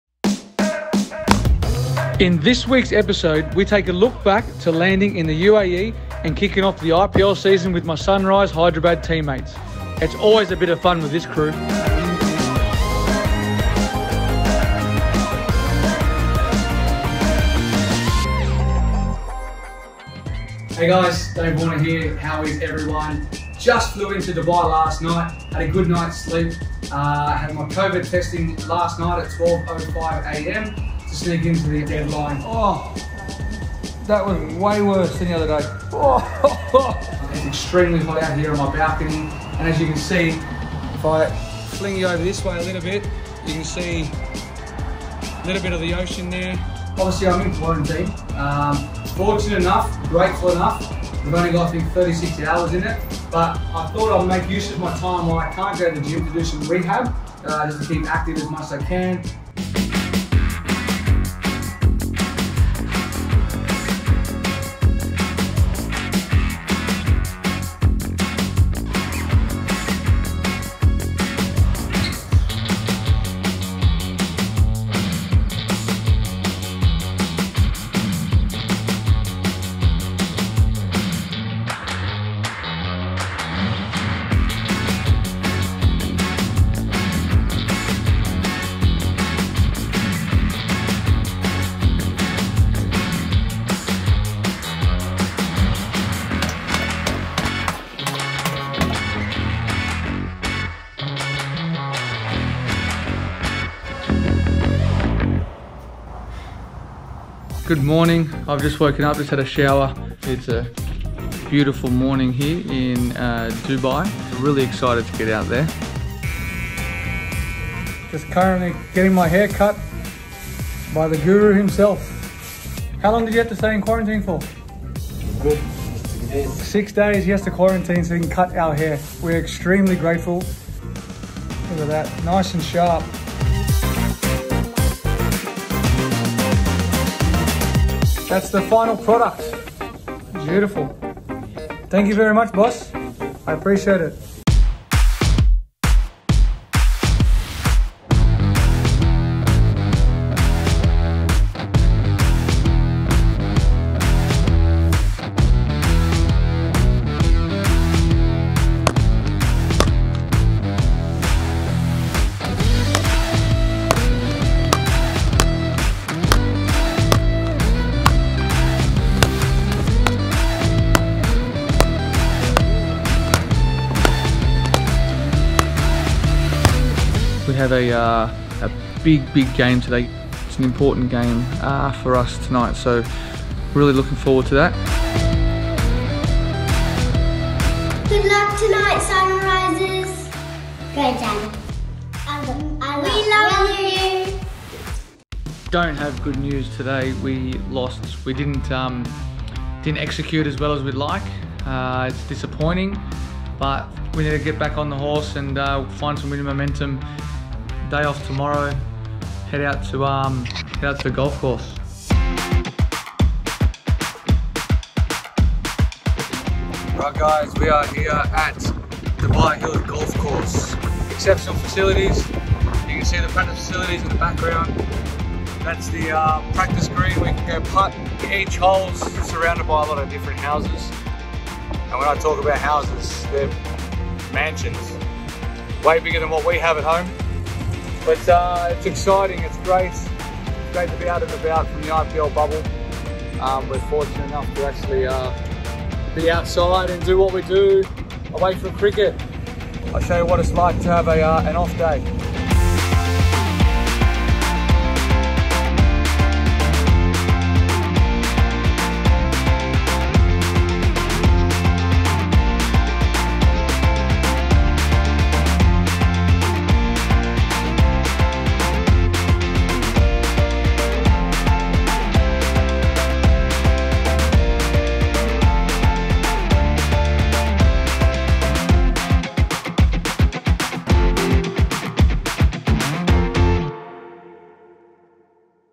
Ep.3 Hello Dubai
Next stop…Dubai! In Ep.3, David lands in the UAE to kick off the IPL season with his Sunrisers Hyderabad team mates. His second quarantine, his fifth COVID-19 test but a bit of fun in the sun and on the golf course makes it all worth it!